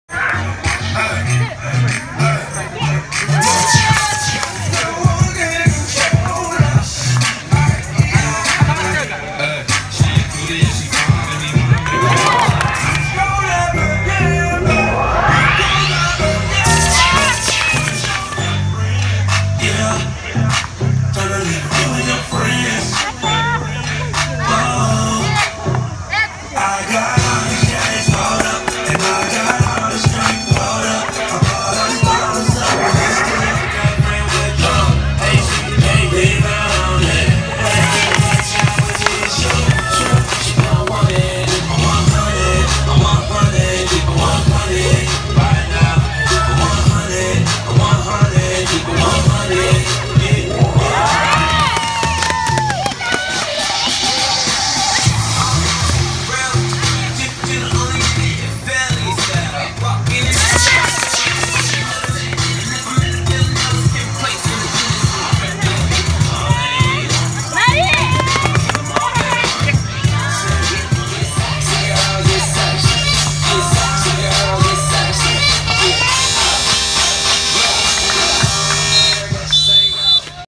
バスケスクール T's Factory 3×3 in両国 2016,8/15 GAME1
バスケスクールT's Factoryが3×3に初参戦！！